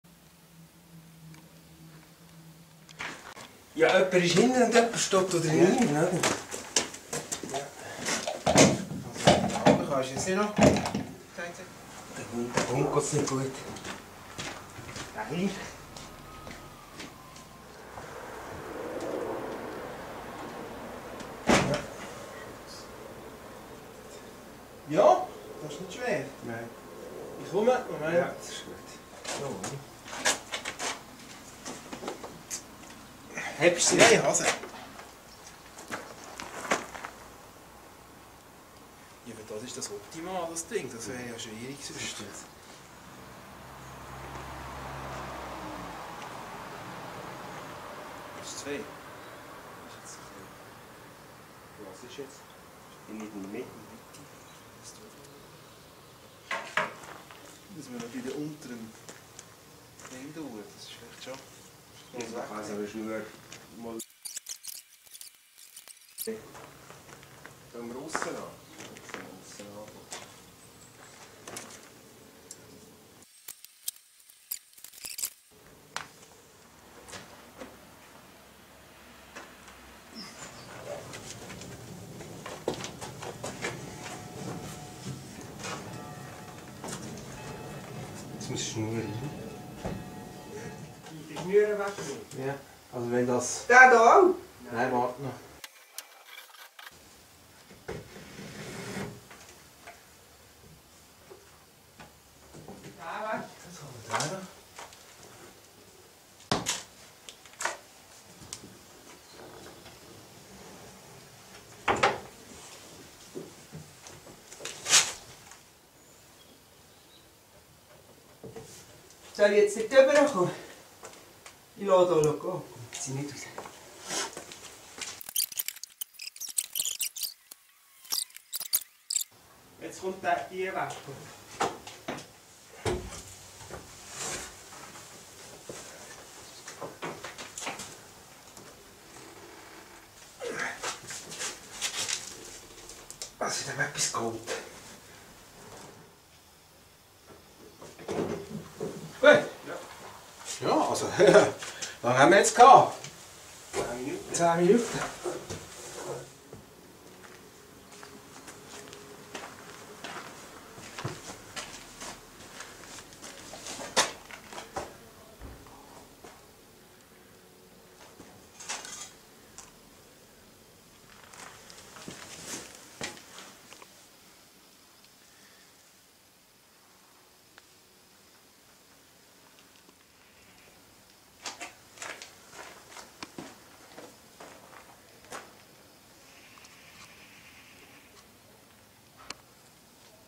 Folge 28 - Einbau der Frontscheibe – der 2. Versuch
Restauration Mercedes 220 (w111), Baujahr 1964: Leider mussten wir feststellen, dass das Einpassen der Chromleiste bei bereits eingebauter Scheibe nicht funktioniert. Wir müssen die Frontscheibe deshalb nochmals herausnehmen und dann zuerst die Chromleise hineindrücken. Erst dann montieren wir Scheibe, Gummi und Chromleiste wieder ans Fahrzeug. Da wir die einzelnen Arbeitsschritte schon gut geübt hatten, ist die Scheibe nach ca. 10 Minuten eingebaut!